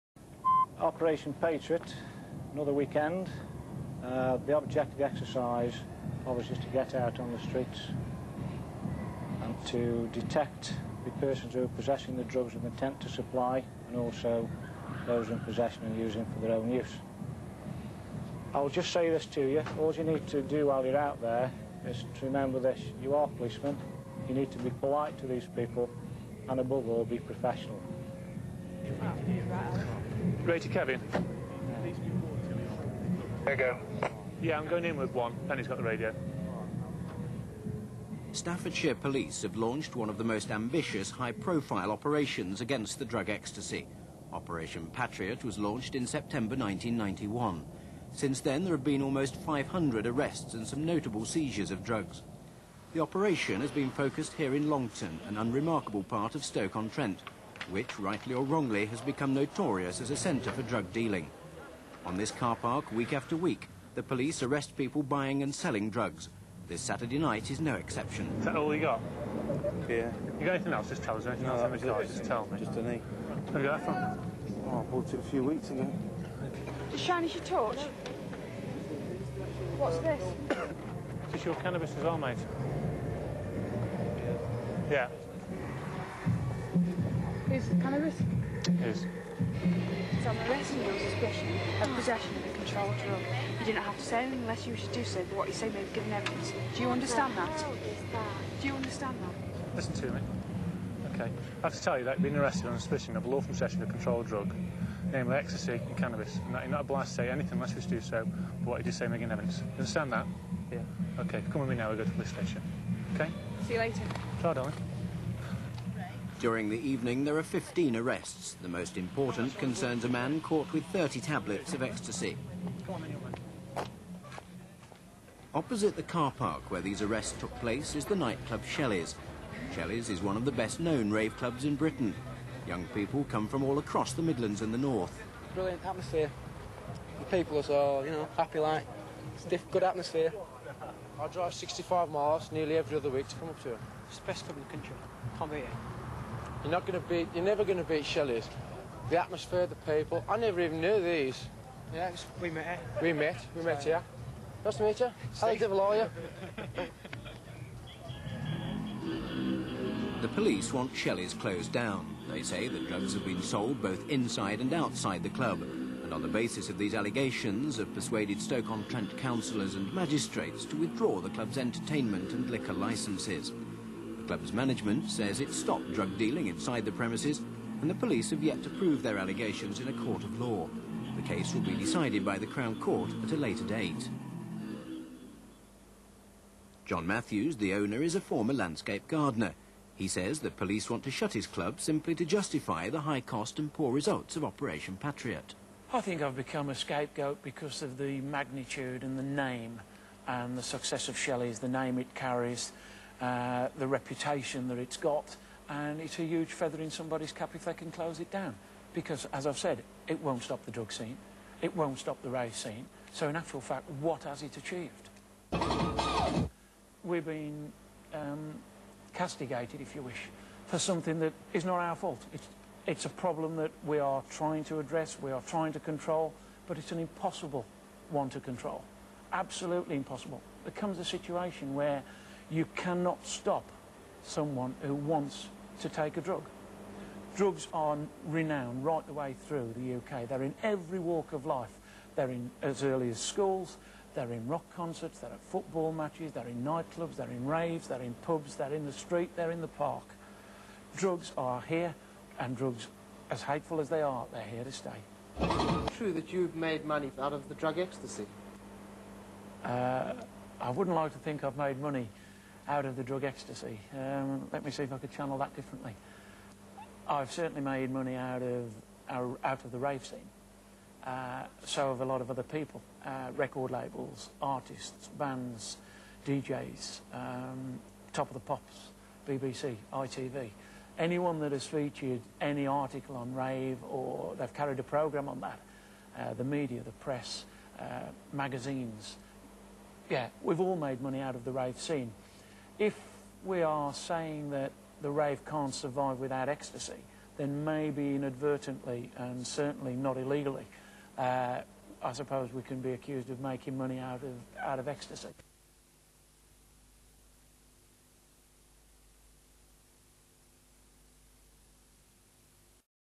Shelleys Bust
Operation Patriot Longton Old Bill Busting Innocent Ravers As They Step Out Of Their Cars On Shelleys Carpark Stoke